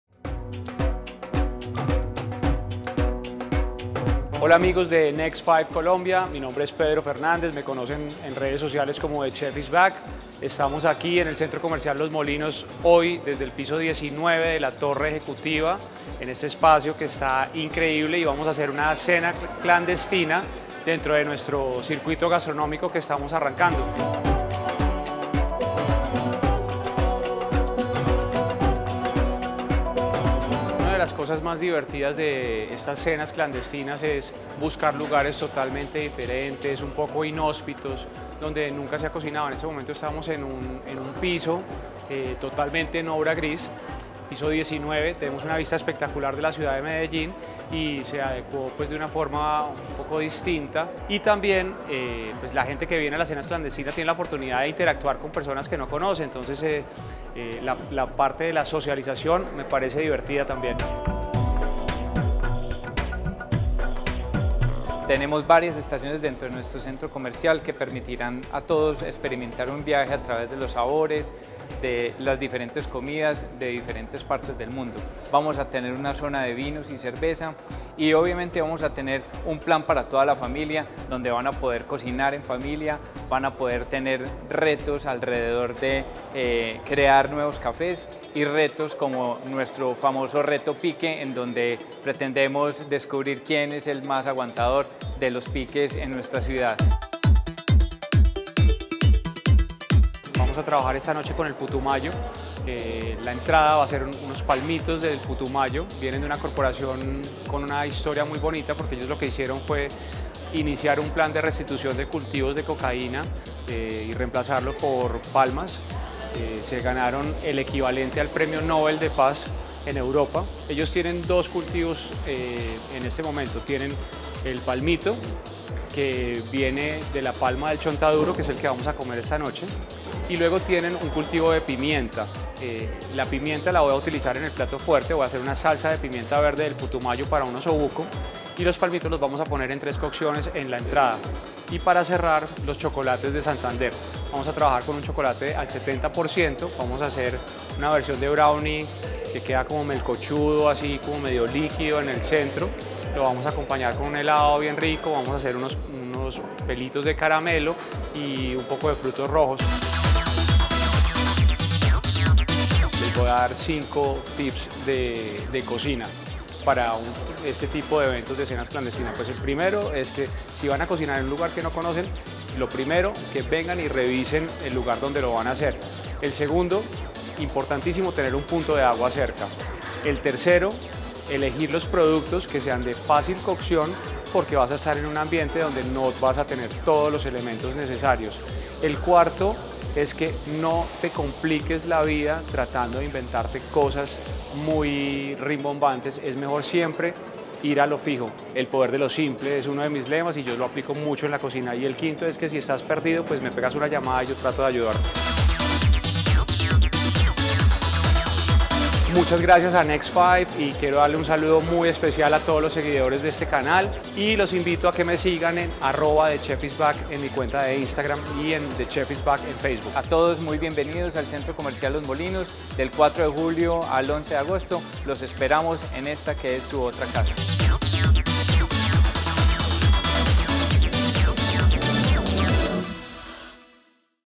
CIRCUITO GASTRONÓMICO Cena Clandestina en OBRA GRIS
Las cenas clandestinas son un fenómeno global que ya arrasa en medio mundo. Se celebran en casas particulares, en lugar de en restaurantes, y ofrecen una experiencia única e irrepetible a los comensales. Participar en una cena clandestina es un pequeño lujo para el que tienes que estar muy atento, porque las convocatorias suelen ser discretas y las plazas muy limitadas.